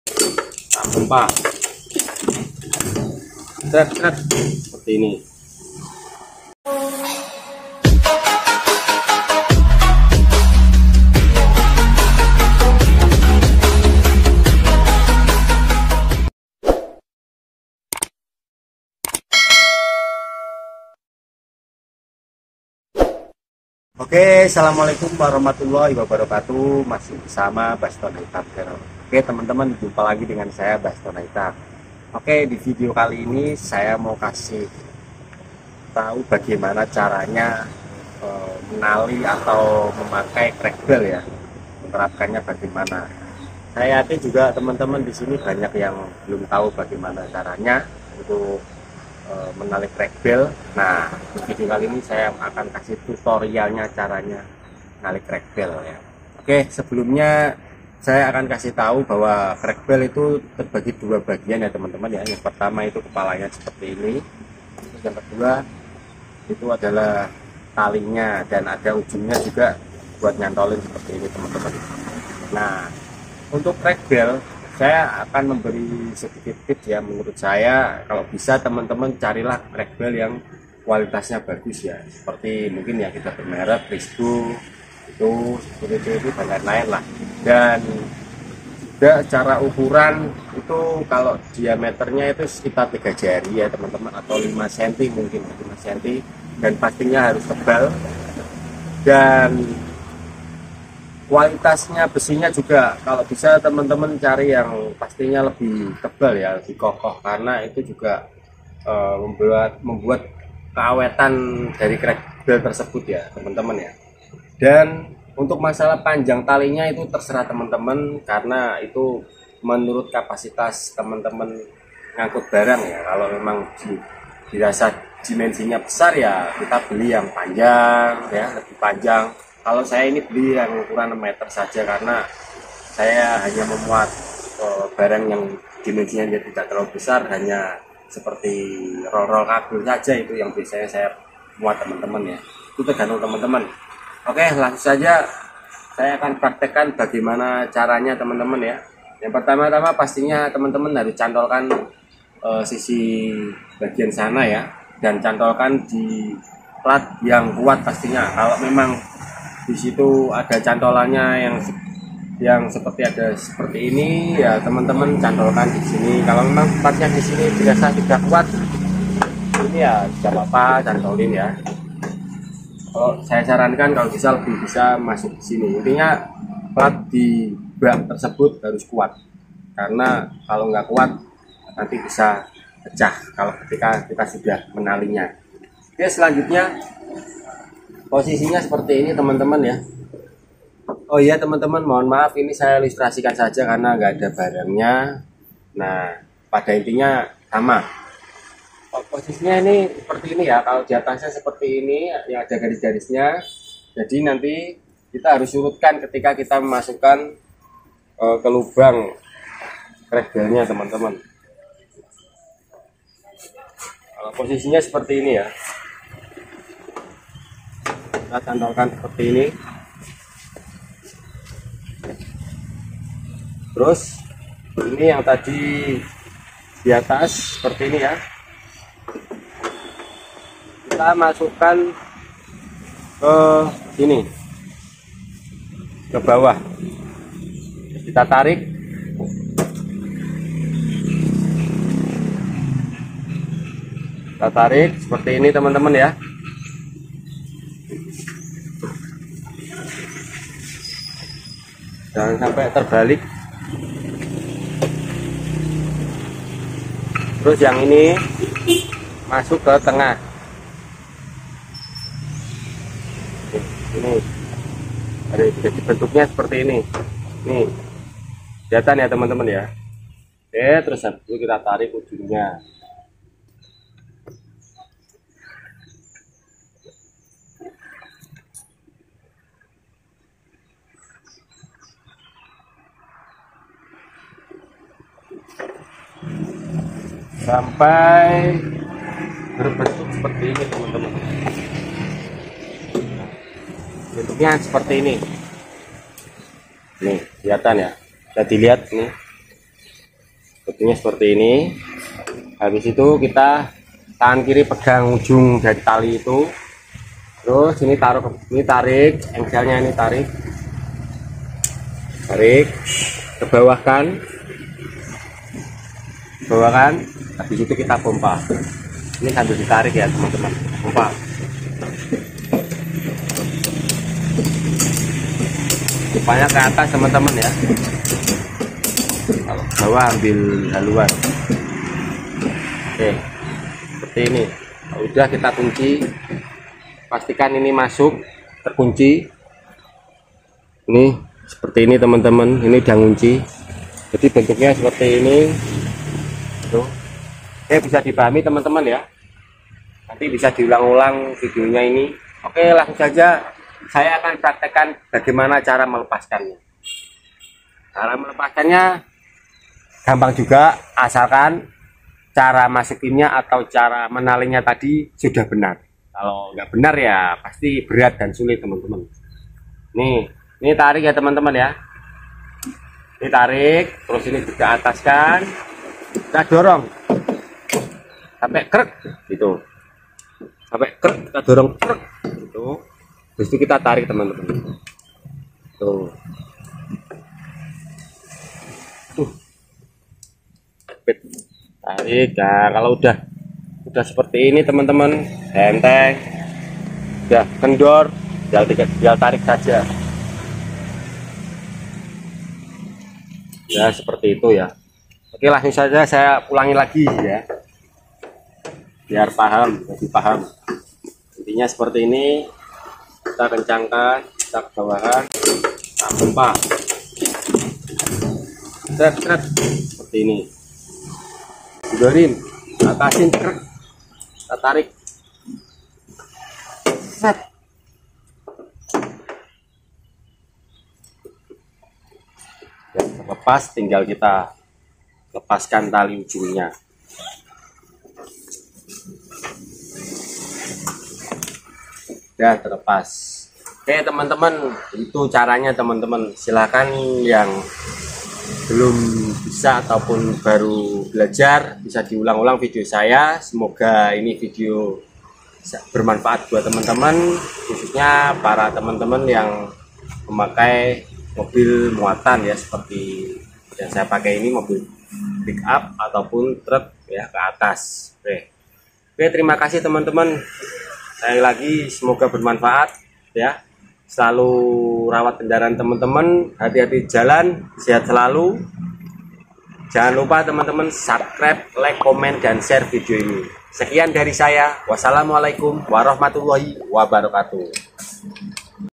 CARA MENGGUNAKAN KREK BELT/TREK BELT
Oke masih bersama BASTON HAITAM CHANNEL

oke temen" di video kali ini saya mau kasih tau bagaimana caranya menali atau menggunakan tali KREK BELT/ TREK BELT dan sebagainya sebutkannya lah😁